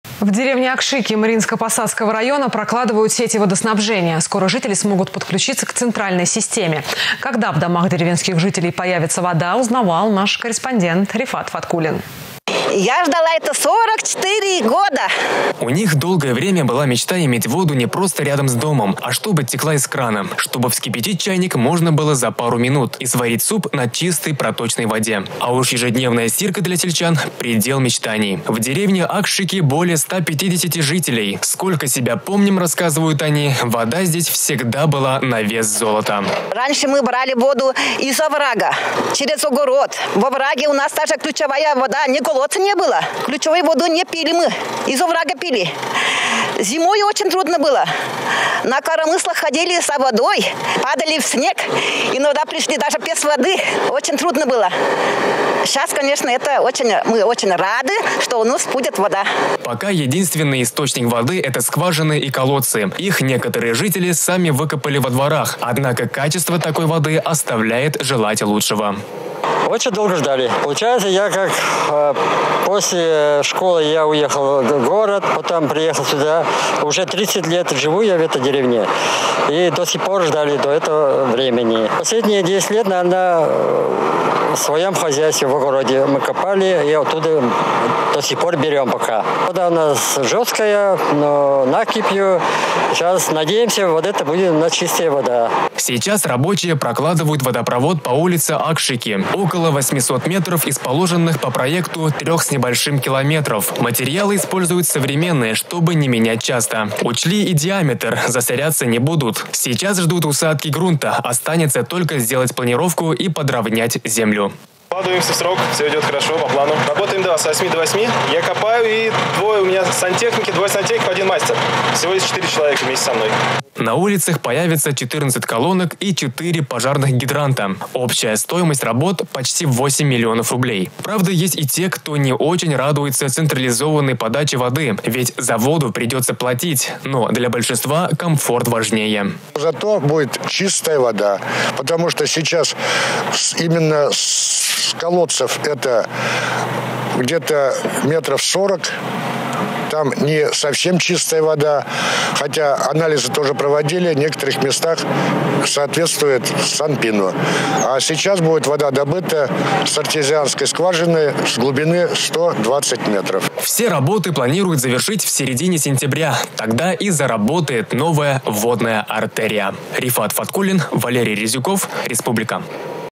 В деревне Акшики Мариинско-Посадского района прокладывают сети водоснабжения.
Скоро жители смогут подключиться к центральной системе.  Когда в домах деревенских жителей появится  вода, узнавал наш корреспондент Рифат Фаткуллин.